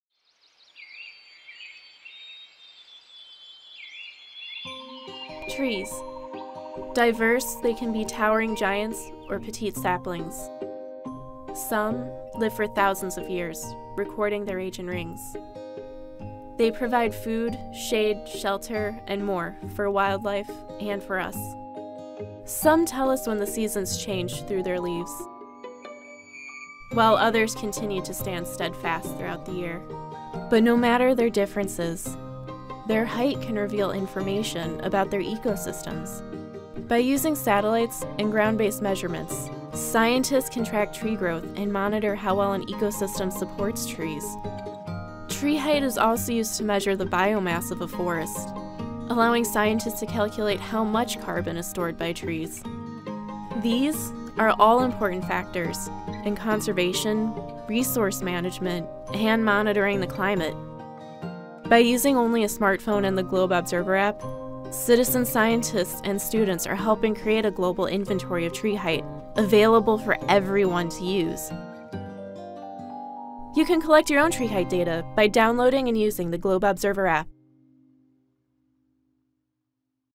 GLOBE Observer: Tree Height
Trees are diverse and tree height can tell us a lot about Earth’s ecosystems. Satellites and ground-based measurements are used to track tree growth, monitor how well an ecosystem supports trees, and estimate how much carbon is stored by trees. GLOBE’s citizen scientist community encourages citizen scientists to use the GLOBE Observer app to take tree height measurements with their smart phones. These observations are added to a freely available, global inventory of tree height. GLOBE Observer also has tools to take observations of clouds, land cover, and mosquito habitats.

Download the GLOBE Observer app and start taking tree height measurements